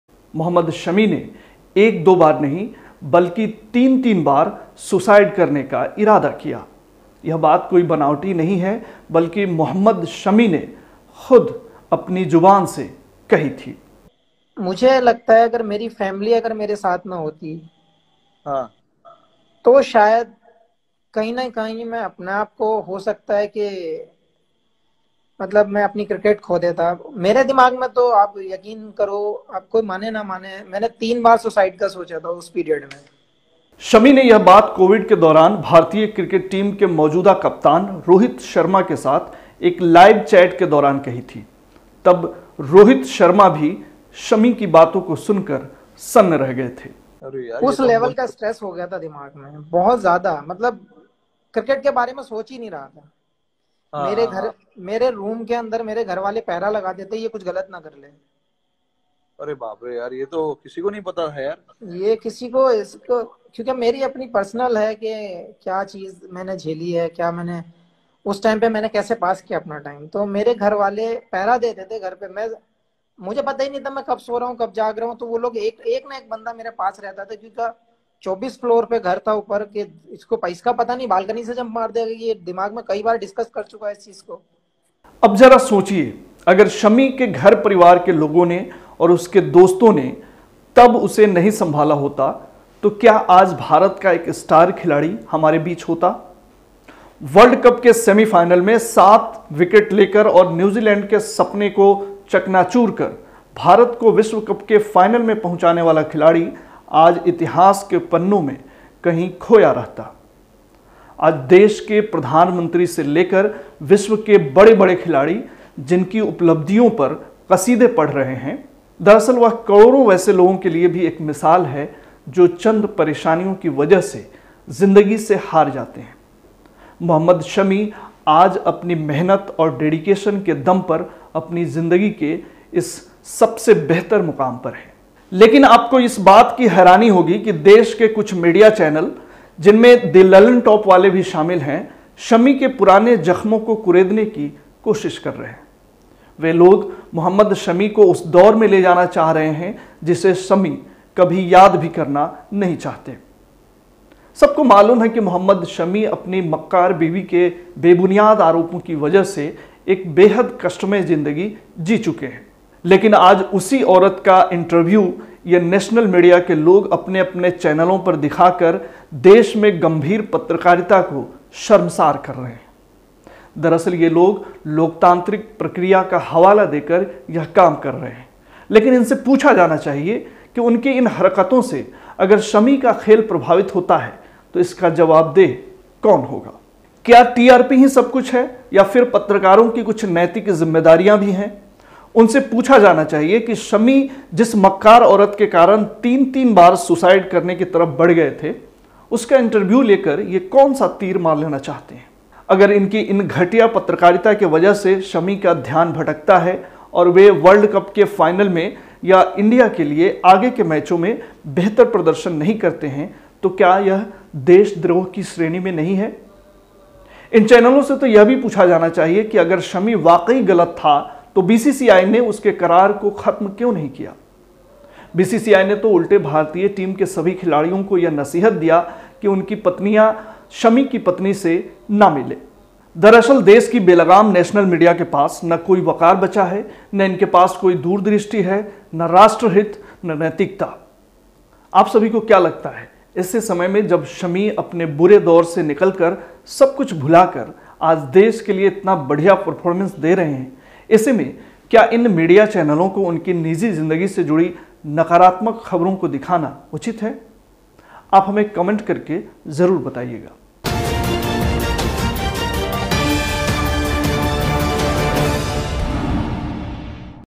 Shami को Suicide के लिए उसका रही है National Media ! इतनी बेशर्म पत्रकारिता क्यों ?
भारतीय टीम के जिस हरफनमौला क्रिकेट खिलाड़ी मोहम्मद शमी की आज पूरी दुनिया मे चर्चा हो रही है । जिस खिलाड़ी पर आज पूरा देश गर्व कर रहा है और सोशल मीडिया से लेकर हर जगह लोगों की जुबां पर दिस खिलाड़ी का नाम है क्या आप जानते हैं एक समय ऐसा भी गुजरा है इस खिलाड़ी के जीवन मे जब वह सुसाइड करने की तरफ बढ़ गया था । मोहम्मद शमी ने एक दो बार नही बल्कि तीन तीन बार सुसाइड करने का इरादा किया । यह बात कोई बनावटी नही बल्कि मोहम्मद शमी ने खुद अपने जुबान से कही है ।

शमी ने यह बात कोविड के दौरान भारतीय क्रिकेट टीम के मौजूदा कप्तान रोहित शर्मा के  साथ एक लाइव चैट के दौरान कही थी ।तब रोहित शर्मा भी हैरान रह गए थे ।
     लेकिन जरा सोचिए अगर अगर शमी के घर परिवार के लोगों ने और उसके दोस्तों ने तब उसे नही संभाला होता तो क्या आज भारत का एक स्टार क्रिकेट खिलाड़ी हमारे बीच होता ? वर्ल्ड कप के सेमिफाईनस मे सात विकेट लेकर और न्यूजीलैंड के सपने को चकनाचूर कर  भारत को  विश्वकप के फाइनल मे पहुंचाने वाला खिलाड़ी आज इतिहास के पन्नों मे कहीं खोया रहता अगर उसके अपनो ने उसके बुरे दौर मे  सहारा न दिया होता।  आज देश के प्रधानमंत्री से लेकर विश्व के बड़े बड़े खिलाड़ी जिसकी उपलब्धियों पर कसिदे पढ़ रहे दरअसल वह करोड़ों वैसे लोगों के लिए भी एक मिसाल हैं जो चंद परेशानियों की वजह से जिंदगी से हार जाते हैं । मोहम्म्द शमी आज अपनी मेहनत और डेडिकेशन के दम पर अपनी जिंदगी के इस मुकाम पर हैं ।
    लेकिन आपको इस बात की हैरानी होगी कि देश के कुछ मीडिया चैनल जिसमे दि लल्लन टॉप वाले भी शामिल है, शमी के पुराने जख्मों को कुरेदने की कोशिश कर रहे हैं। वे लोग मोहम्मद शमी को उस दौर मे ले जाना चाह रहे हैं जिसे शमी कभी याद भी करना नही चाहते  । सबको मालूम है कि मोहम्मद शमी अपनी मक्कार बीवी के बेबुनियाद आरोपों की वजह से एक बेहद कष्टमय जिंदगी जी चुके हैं । लेकिन आज उसी औरत का इंटरव्यू ये नेशनल मीडिया के लोग अपने-अपने चैनलों पर दिखाकर गेश मे गंभीर पत्रकारिता को शर्मसार कर रहे हैं । दरअसल ये लोग लोक लोकतांत्रिक प्रक्रिया का हवाला देकर यह काम कर रहे हैं लेकिन इनसे पूछा जाना चाहिए कि उनकी इन हरकतों से अगर शमी का खेल प्रभावित होता है, तो इसका जवाबदेह कौन होगा ?  क्या TRP हीं सबकुछ है या फिर पत्रकारों की कुछ नैतिक जिम्मेदारियां भी हैं ? उनसे पूछा जाना चाहिए कि शमी जिस औरत के कारण तीन तीन बार सुसाइड करने की तरफ बढ़ गए थे उसका इंटरव्यू लेकर ये कौन सा तीर मार लेना चाहते हैं ?अगर इनकी इन घटिया पत्रकारिता के वजह से शमी का ध्यान भटकता है और वे वर्ल्ड कप के फाइनल मे या इंडिया के लिए आगे को मैचों मे बेहतर प्रदर्शन नही करते हैं तो क्या यह देशद्रोह की श्रेणी मे नही है ?
        दरअसल देश की बेलगाम नेशनल मीडिया के पास न कोई वकार बचा है , न इनके पास कोई दूरदृष्टि न राष्ट्रहित न नैतिकता ।
      आप सभी को क्या लगता है ऐसे समय मे जब शमी अपने बुरे दौर से निकल कर सबकुछ भुला कर आज देश के लिए इतना बढिया परफॉरम्मेंस दे रहे हैं , ऐसे मे क्या इन मीडिया चैनलों को उनकी निजी ज़िंदगी से जुड़ी नकारात्मक खबरों को दिखाना उचित है ? आप हमें कमेंट कर के बताइएगा जरूर ।